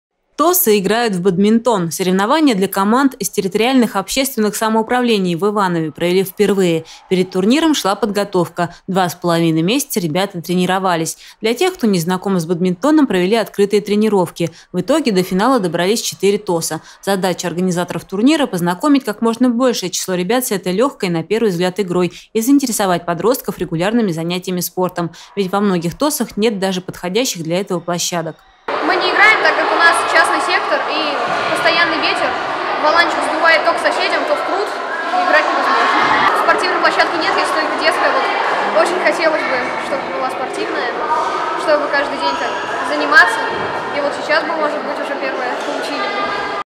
ТОСы играют в бадминтон
ТОСы играют в бадминтон. Соревнования  для команд из территориальных общественных самоуправлений в Иванове провели впервые.